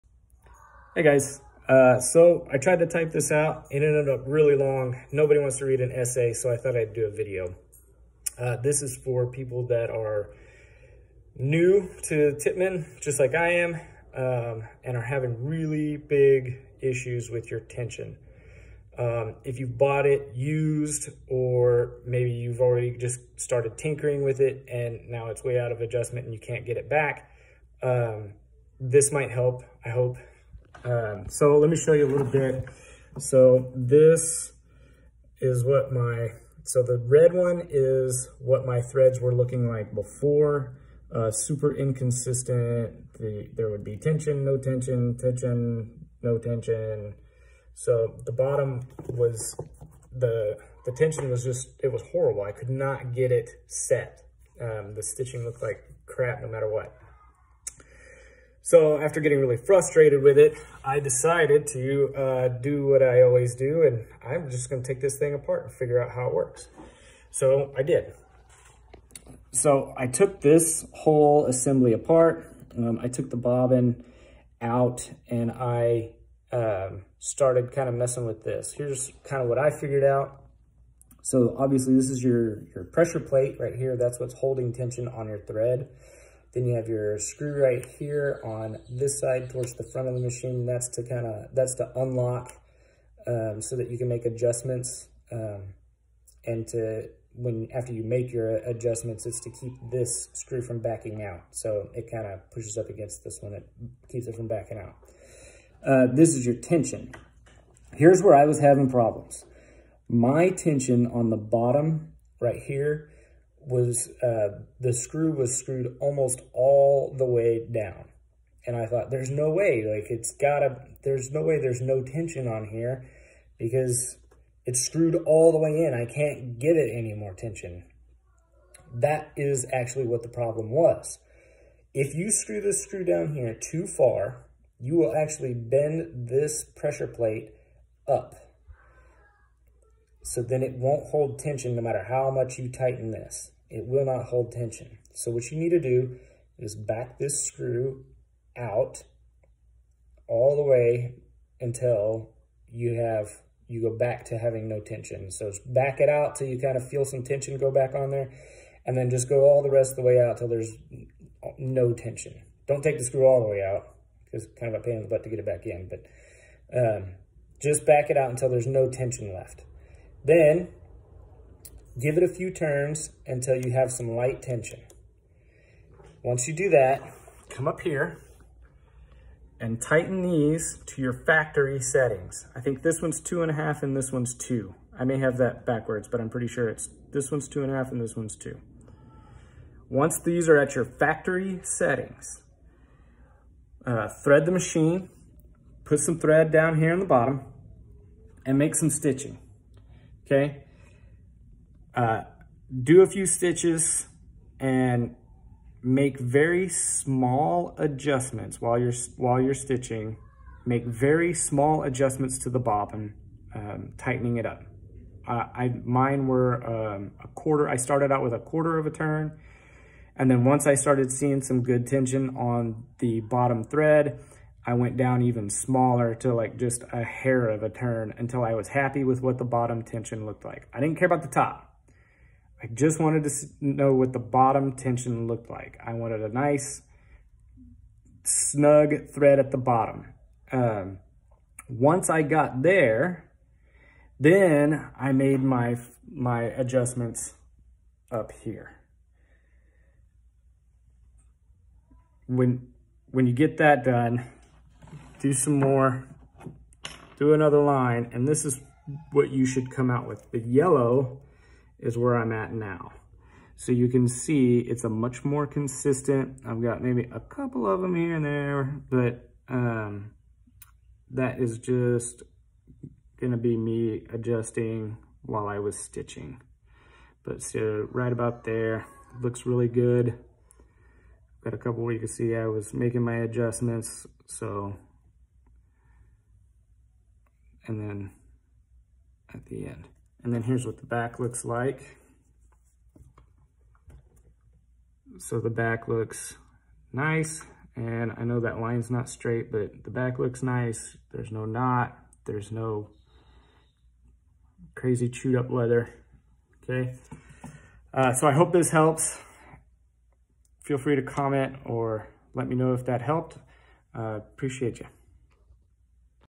Tippmann Boss tension frustration solved!
This video is for anyone having a hard time getting your tension set for your Tippmann Boss. If you bought it used or if you have tinkered to a point that you can’t correct it maybe this video will help. I was very frustrated with my machine and was starting to regret my purchase but now I’m sewing on this thing with ease because of a few small adjustments. I hope I’m able to help!